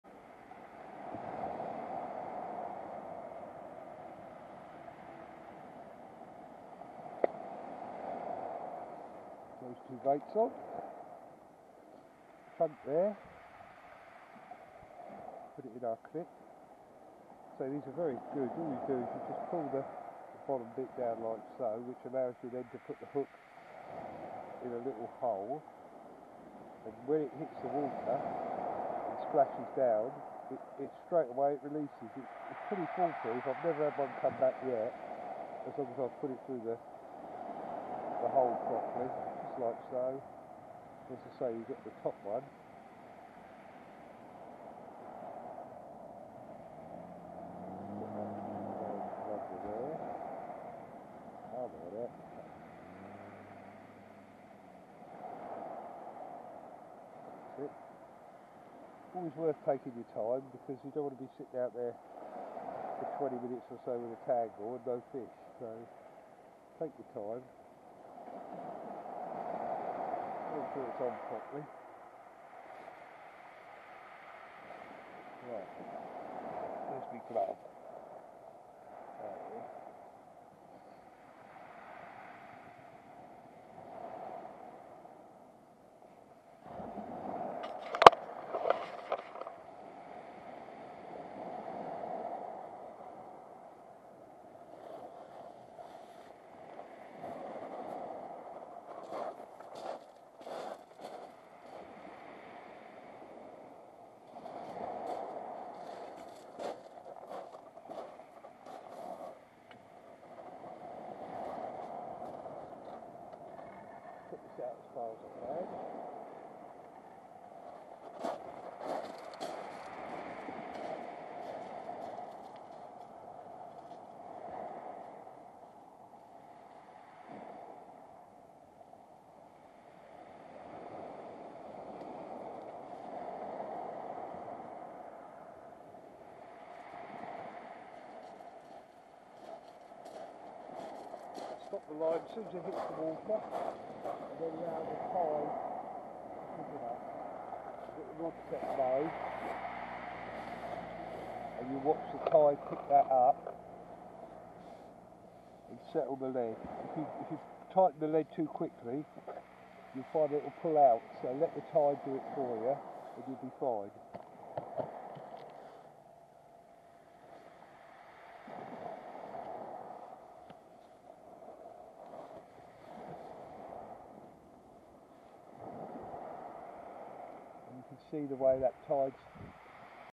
Hythe session part four
The Portsmouth rig did the buisness and sandeel on a size one scratch and match hook